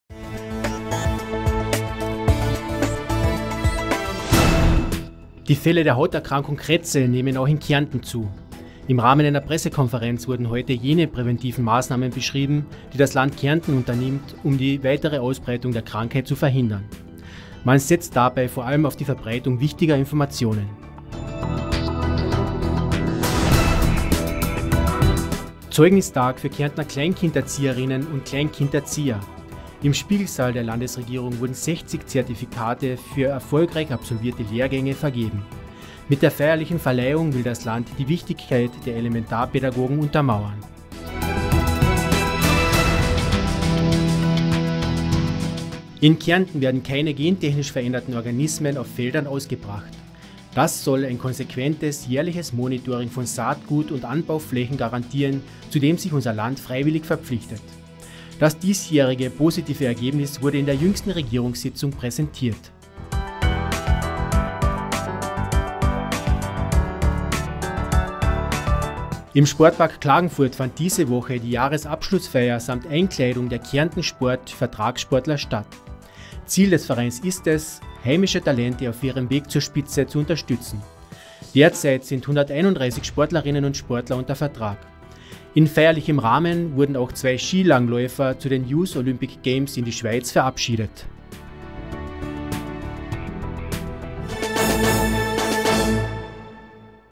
KÄRNTEN IST GENTECHNIKFREI - kärnten.tv90 - 2019/72
Jede Woche die wichtigsten Neuigkeiten aus der Kärntner Landesregierung in 90 Sekunden zusammengefasst. Die Themen heute: Prävention bei Hauterkrankung, Zertifikatsverleihung, Kärnten ist gentechnikfrei und sportlicher Jahresabschluss.

Land Kärnten goes Social Media! Wir, der Landespressedienst, möchten diesen Auftritt für Videos und Statements unserer Regierungsmitglieder und MitarbeiterInnen verwenden.

▬ Social Media ▬▬▬▬▬▬▬▬▬▬▬▬▬▬▬